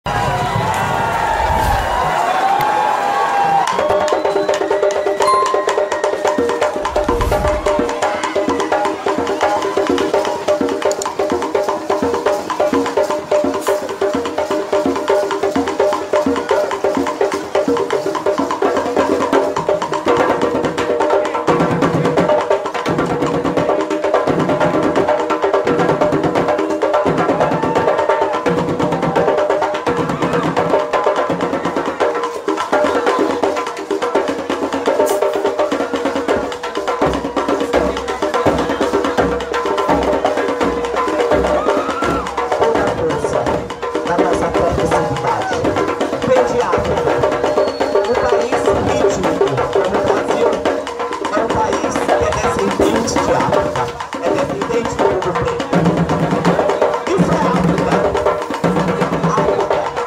Tambores de África | Banda Show
tambores Africano, Parte do espetáculo de apresentação da Banda Show Integração da Bahia , Brasil